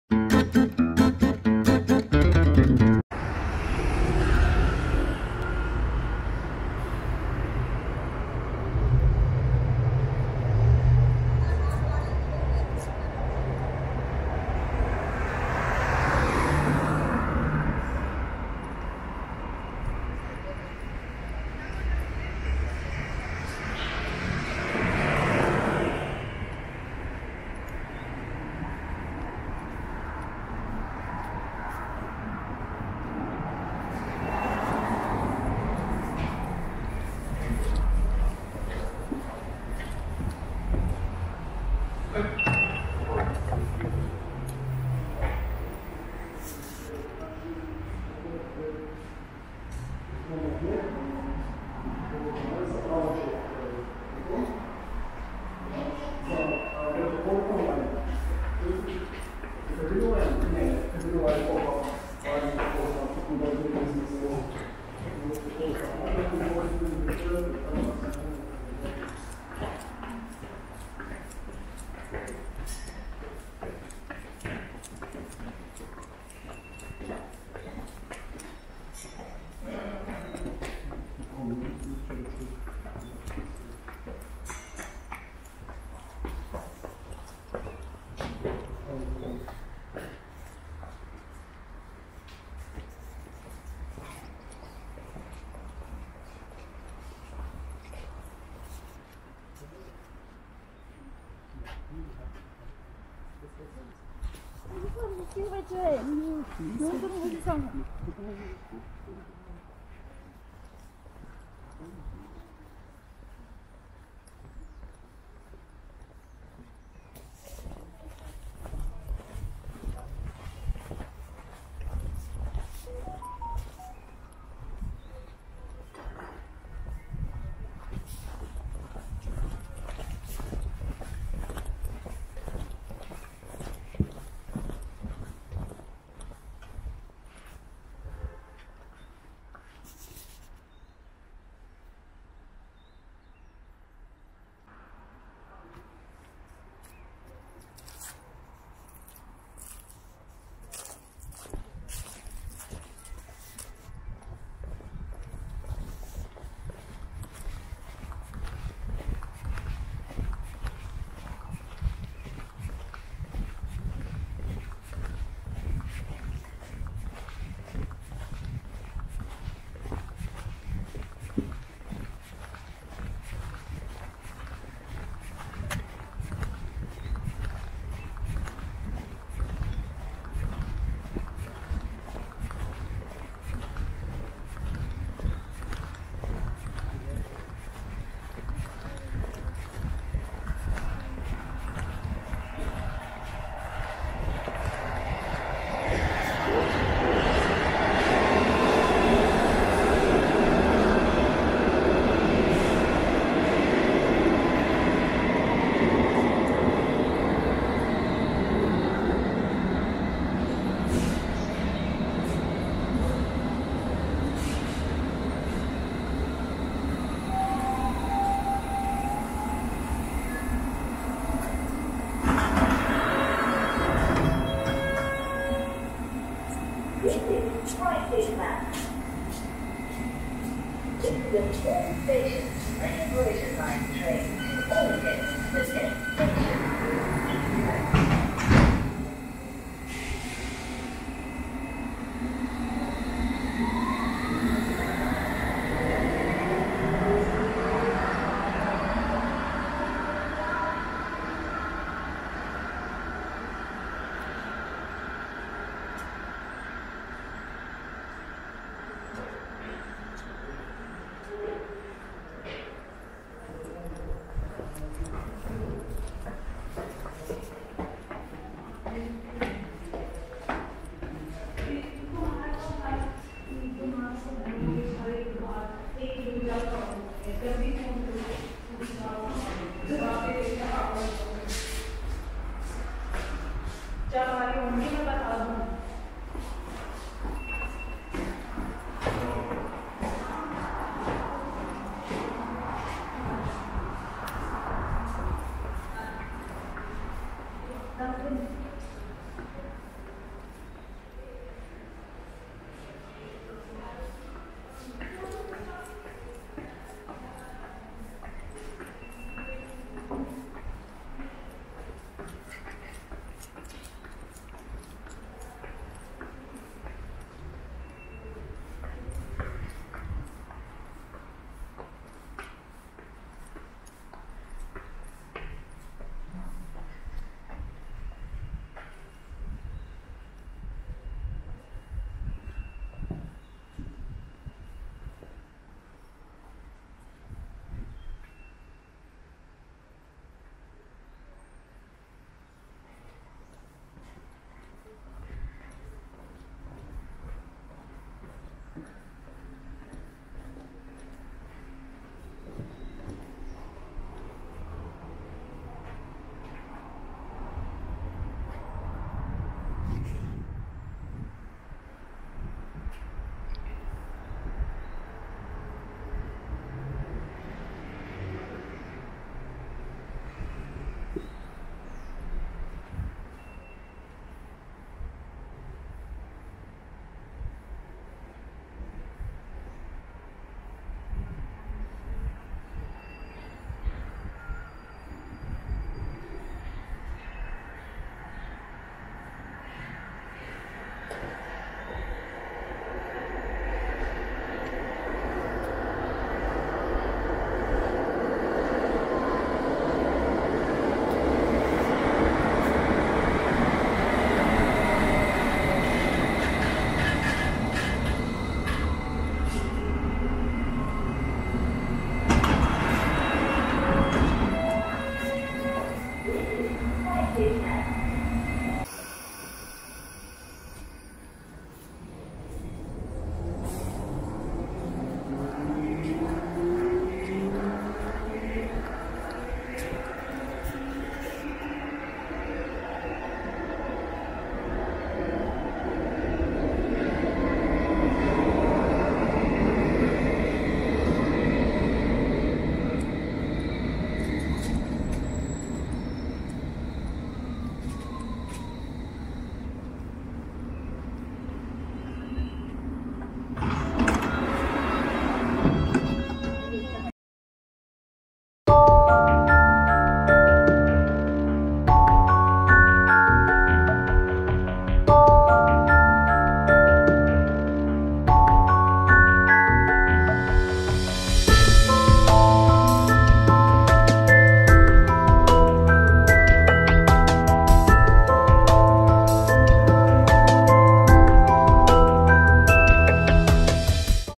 Tube Station Ruislip Manor - London 🇬🇧 - Walkthrough 🚶
Ruislip Manor is a tube station on the Metropolitan and Piccadilly lines.
The station opened on August 5, 1912.